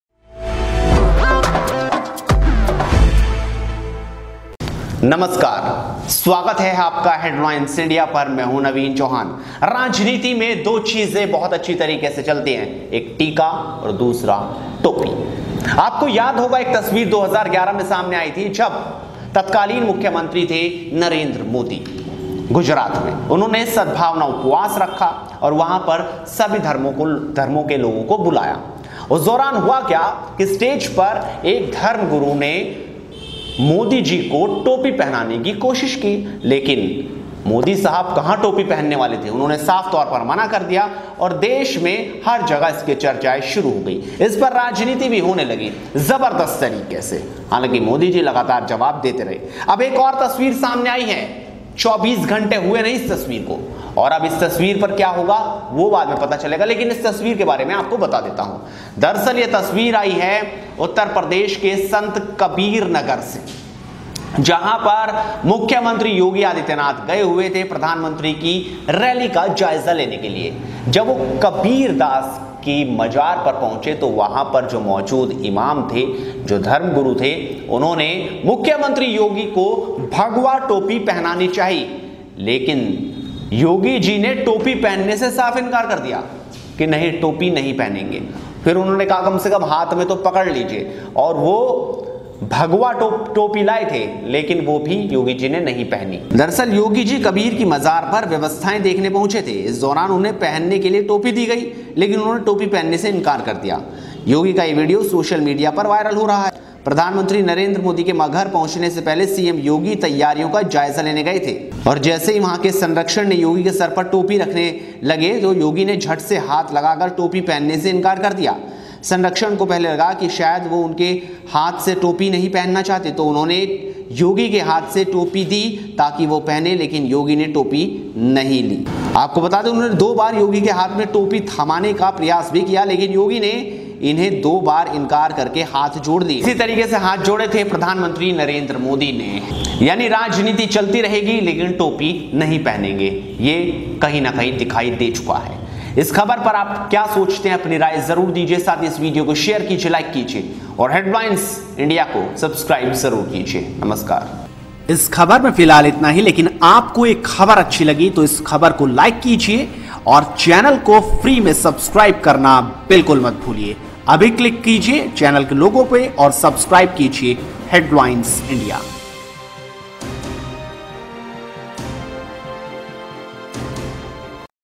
Yogi के सिर पर रखनी चाही मुस्लिम टोपी, तो Yogi ने कर दिया ऐसा की... | Headlines India
Yogi के सिर पर रखनी चाही मुस्लिम टोपी, तो Yogi ने कर दिया ऐसा की...